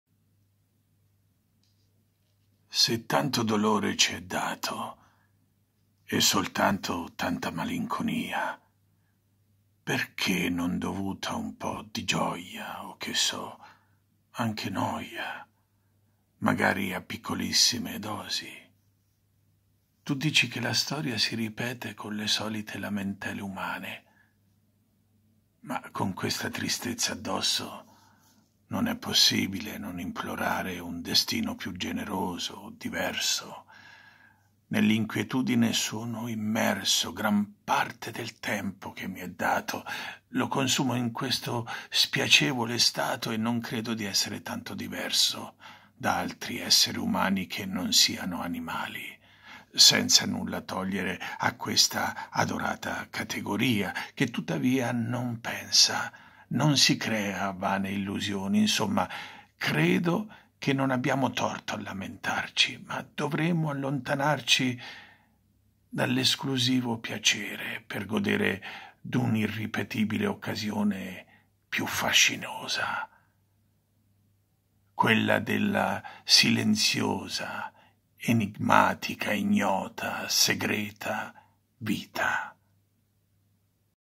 Tu dici che la storia si ripete - Testo e voce di sergio e Basta!
Tu dici che la storia si ripete

Se tanto dolore ci è dato
e soltanto tanta malinconia
perché non dovuta un po' di gioia
o che so di pace o anche noia
magari a piccolissime dosi?
con le solite lamentele umane
ma con questa tristezza addosso
non è possibile non implorare
un destino più generoso diverso
nell'inquietudine sono immerso
gran parte del tempo che mi è dato
lo consumo in questo spiacevole stato
e non credo di essere tanto diverso
da altri esseri umani che non siano animali
senza nulla togliere a questa adorata categoria
che tuttavia non pensa non si crea vane illusioni
insomma credo che non abbiamo torto a lamentarci
ma dovremmo allontanarci dall'esclusivo piacere
per godere d'un irripetibile occasione più fascinosa
quella della silenziosa enigmatica ignota segreta vita.

sergio e Basta!